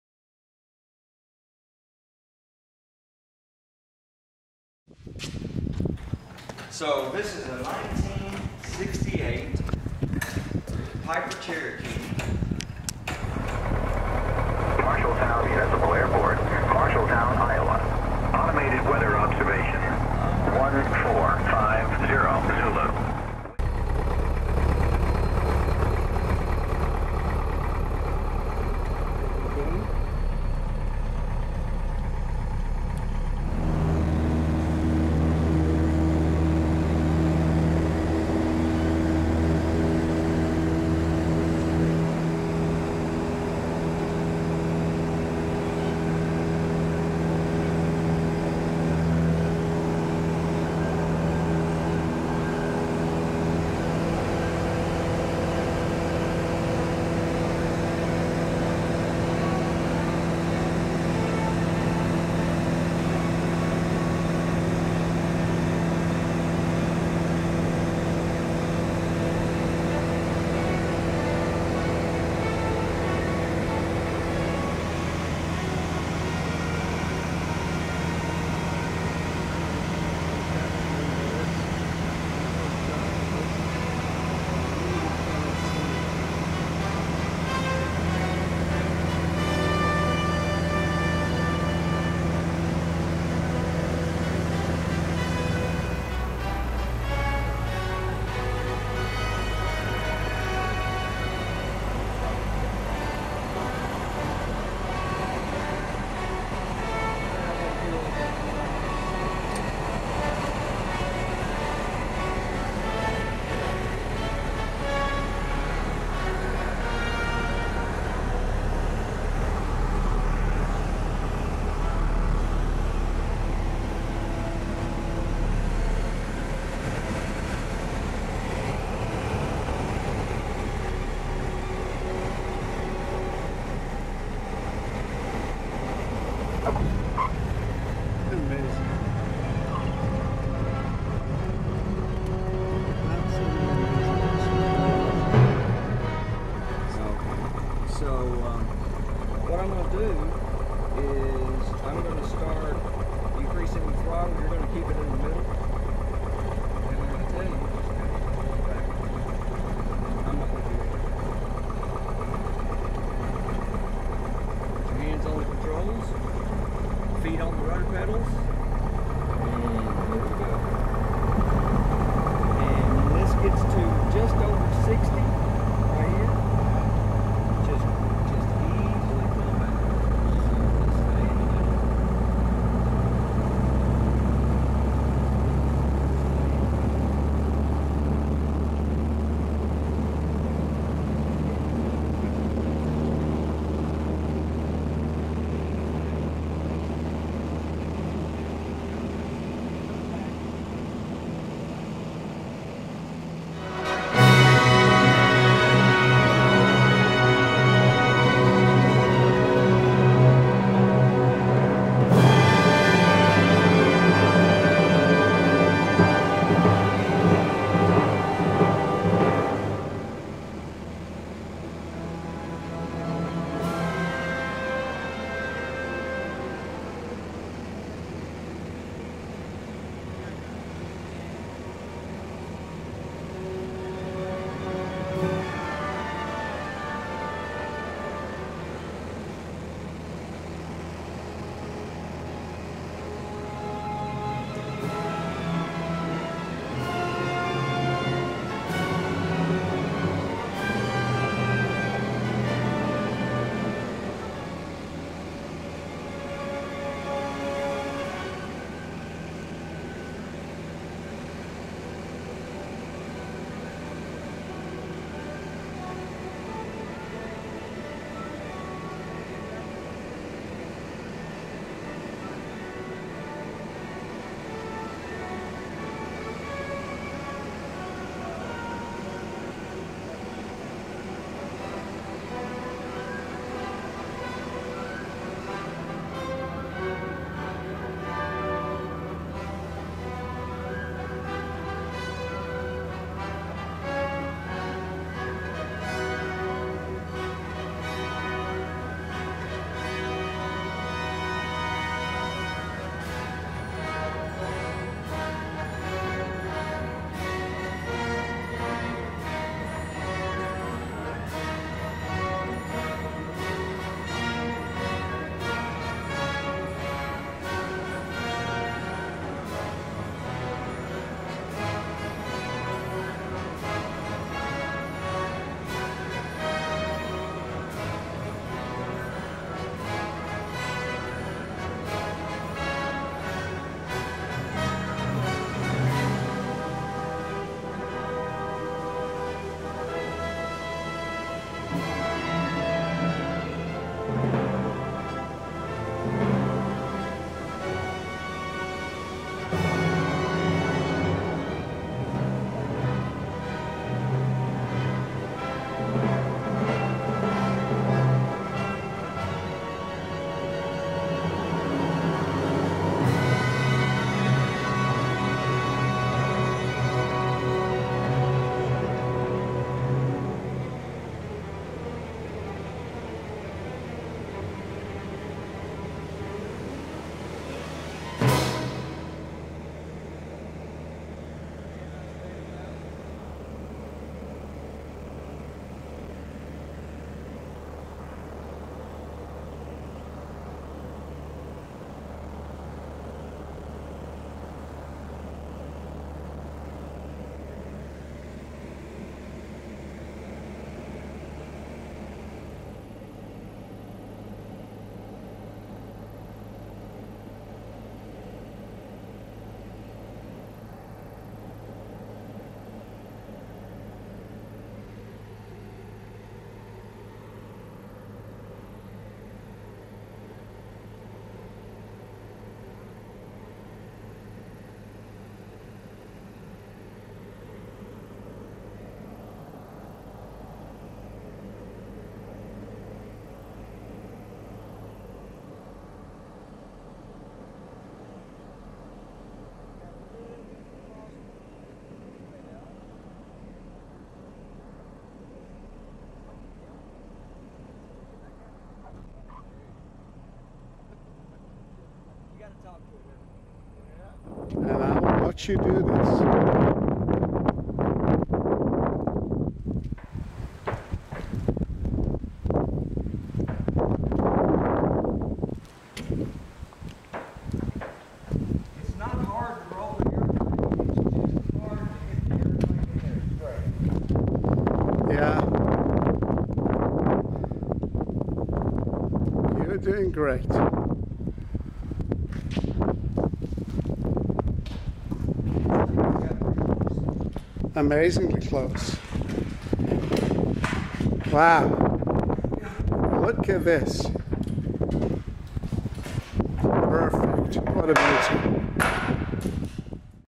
Flight over Iowa
Branko Pecar and I flew N1537J from Marshalltown to Belle Plaine, Iowa to buy fuel.  On the trip back Branko did the take-off and flew the plane most of the way.  I think he liked it.  What do you think?